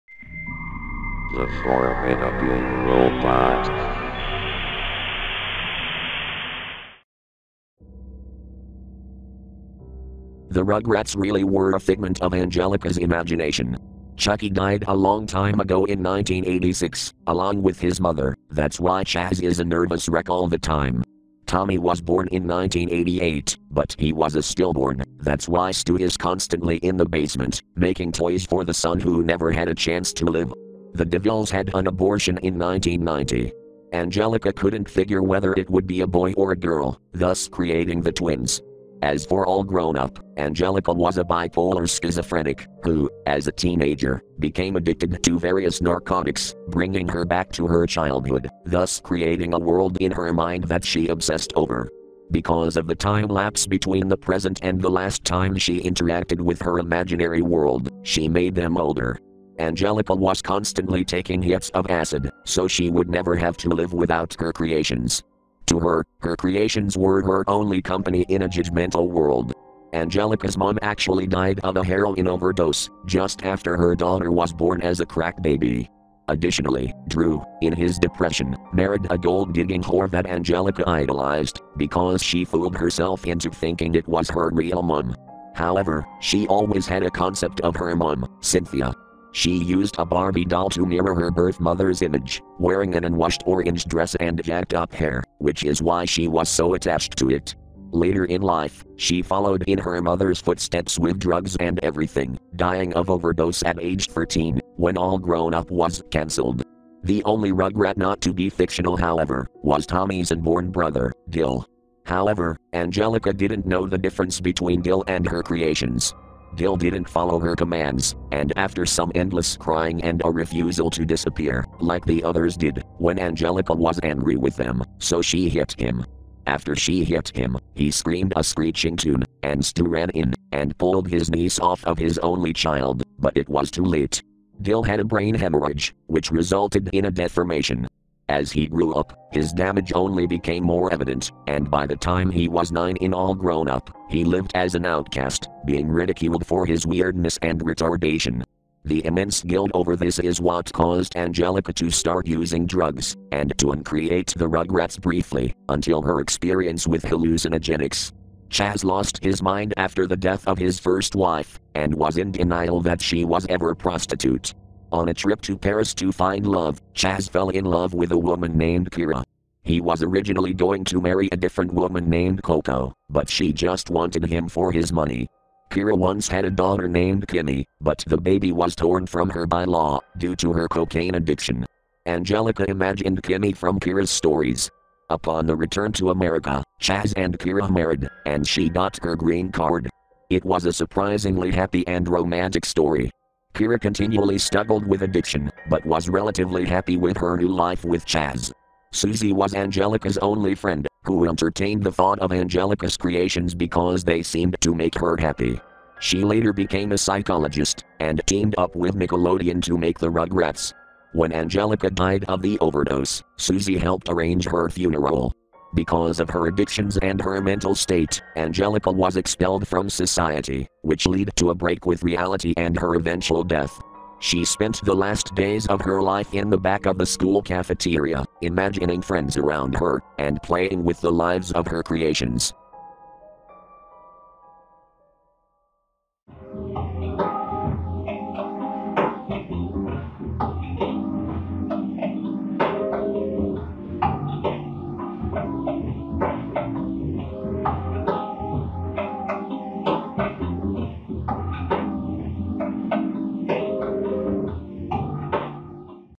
(Creepypasta/Theorypasta) The Rugrats Theory
WARNING: The story may vary in mature, graphic, and gratuitous content.

Credit: Anonymous (possibly DollParts?)

Sound FX from:
Chosic
Disney Wiki
FindSounds
FreeSFX
Freesound
SoundBible
Sounddogs
Soundeffects Wiki
Sound Ideas
Sound Image
Storyblocks
The Sounds Resource
Videvo